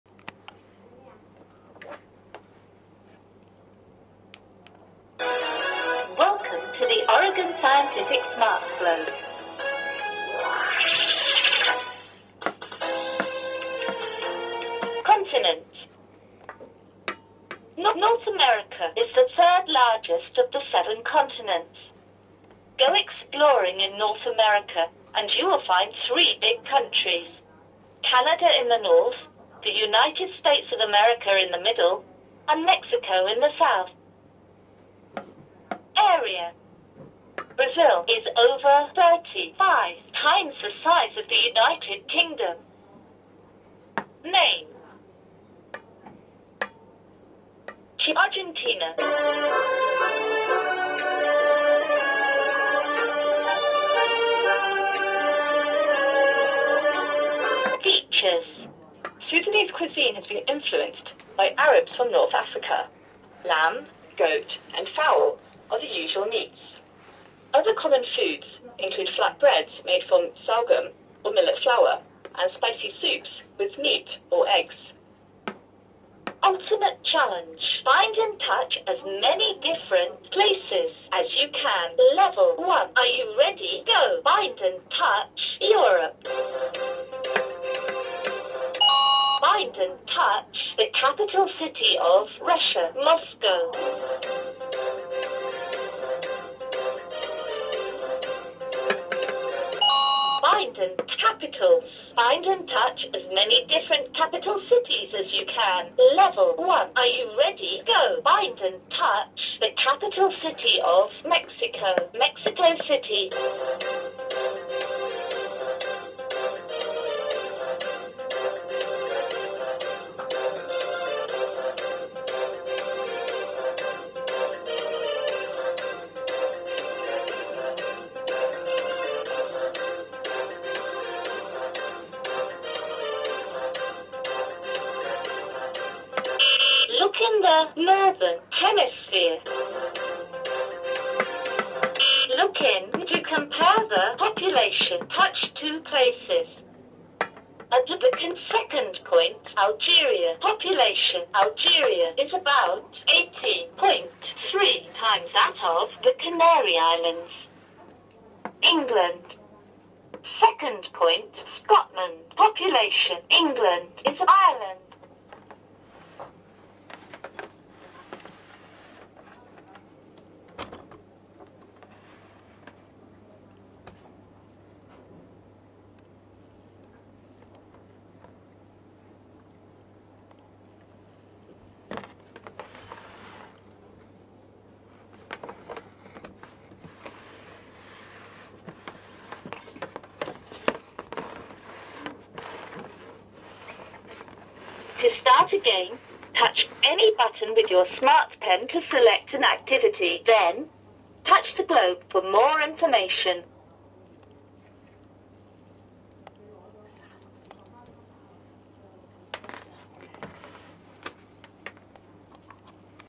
[Oregon Scientific SmartGlobe™ 點讀學習地球儀]
讓小朋友輕鬆認識世界
點讀知識英文發音～真正play to learn

19種不同學習活動
探索超過250個國家的地理、人口、語言、貨幣等知識
3面遊戲板面包括英國地圖
超過2.5小時的audio教育內容
設有挑戰遊戲模式
超過2000種學習知識

適合6歲以上兒童
組裝尺寸：28.3L x 39.9W x 29.7H cm
電池需要：3 x AA電池（隨機附送）